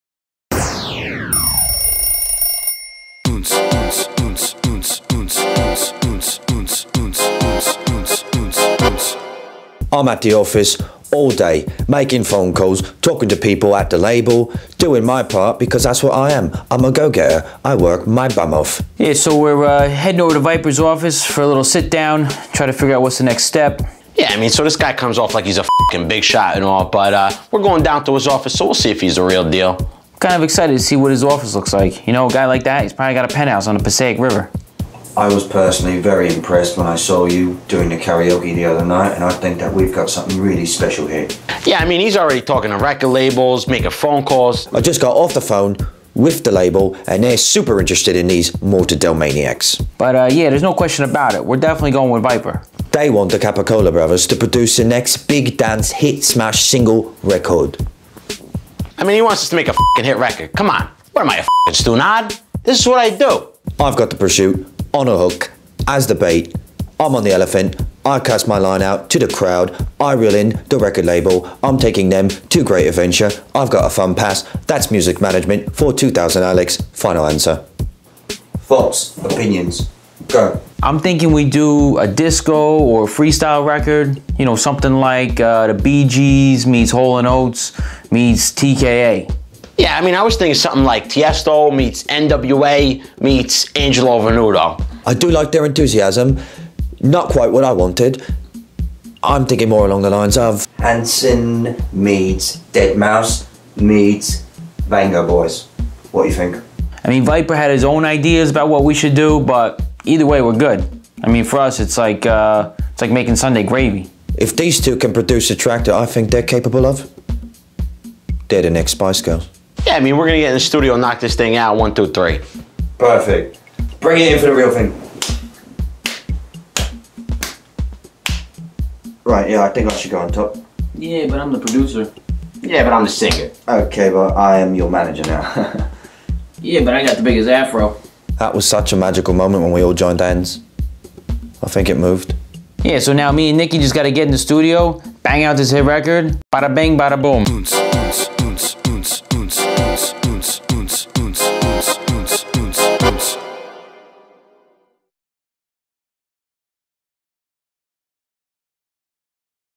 Behind The Meat with The Capicola Brothers (Clip 5)
Meet NJ's hottest dance music duo, The Capicola Brothers.